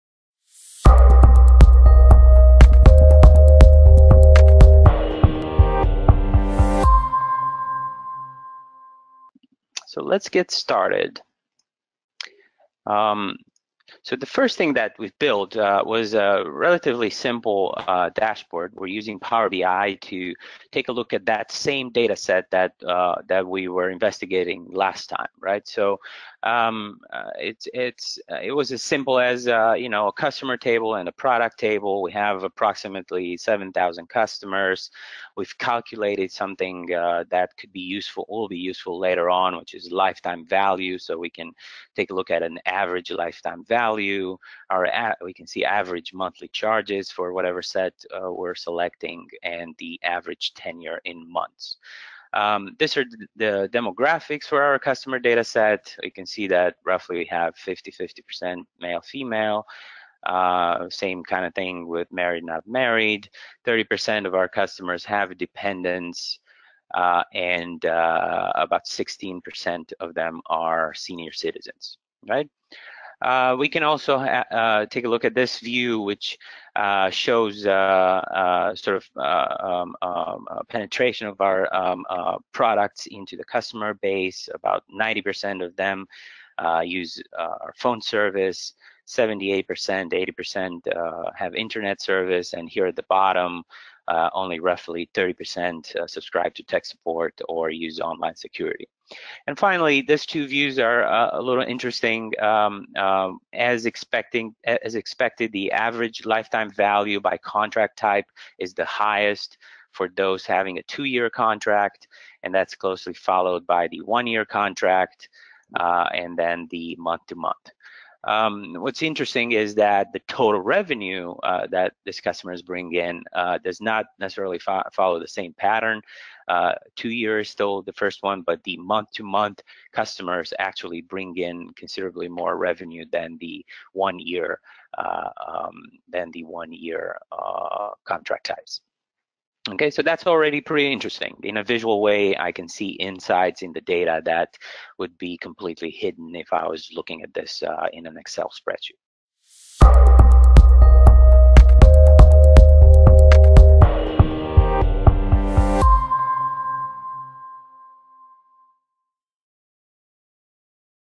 Data Exploration using Microsoft Power BI
Using demographics and product mix insight, Mike Druta performs data exploration with the help of Microsoft Power BI.

Watch the entire demo series in our webinar Opertionalizing Customer Analytics With Power BI